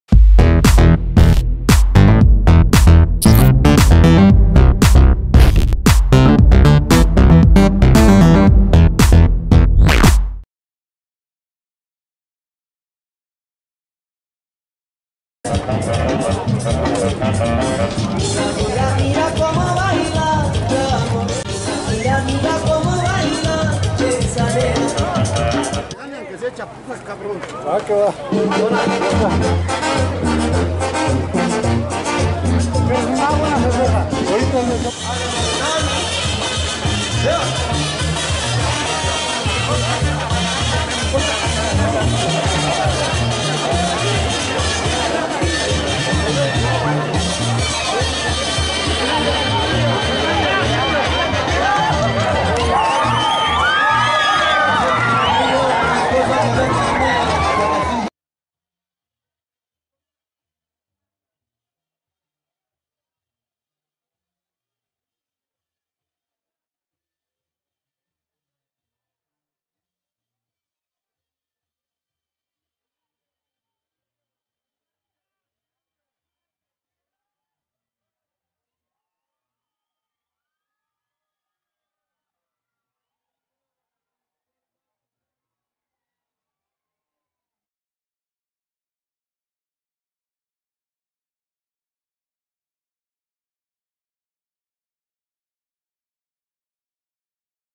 LA SECRETARIA CUADRA AYALA VS EL RETINTO CUADRA GARCIA VS EL H100 CUADRA PURO VIEJON 200 METROS
LA SECRETARIA CUADRA AYALA VS EL RETINTO CUADRA GARCIA VS EL H 100 CUADRA PURO VIEJON 200 METROS

DOMINGO 13/AGOSTO/2023 CARRIL EL VICARIO DE APASEO EL GRANDE GTO.